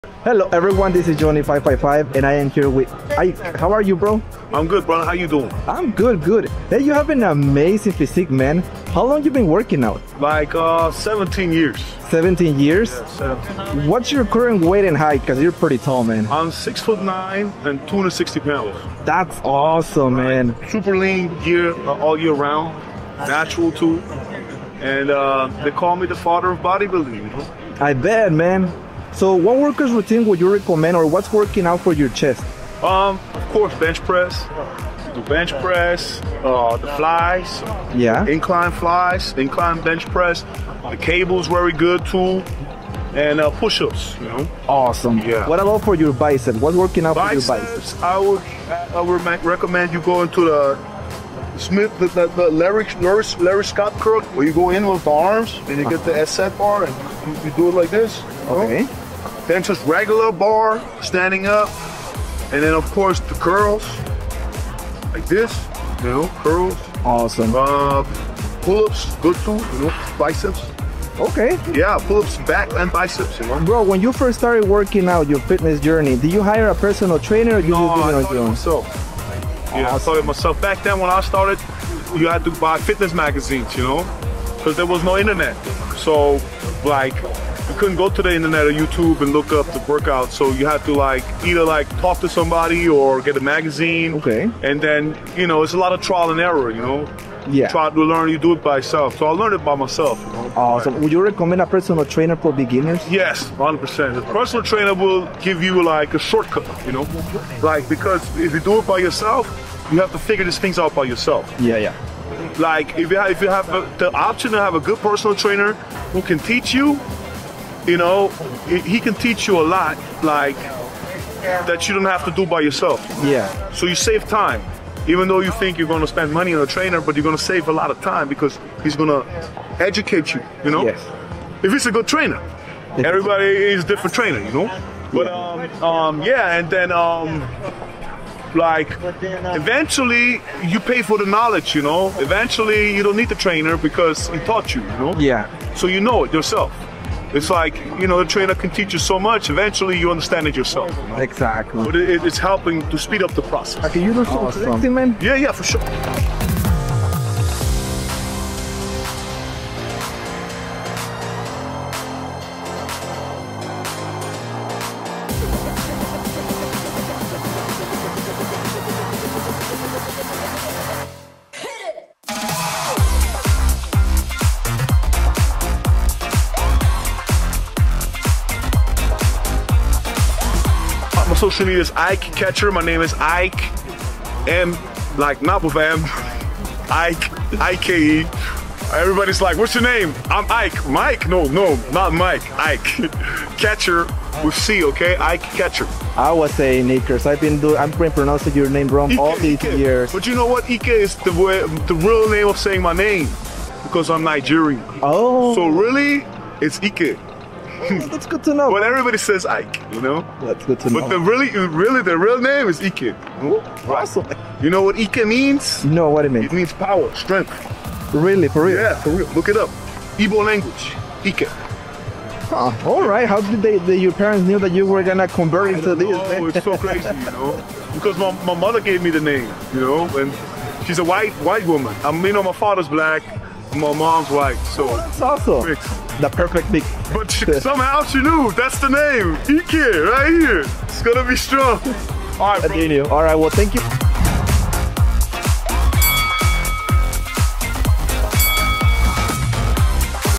Interviewing 6ft9 Ike Catcher the tall guy at muscle beach
Interviewing the one and only ​⁠@ikecatcher7722 💪🏼💪🏼💪🏼 the tall king of muscle beach in LAX (Los Angeles CA) venice beach. If you want to see more of tall Ike follow his channel which is tagged. 💪🏼😉🎥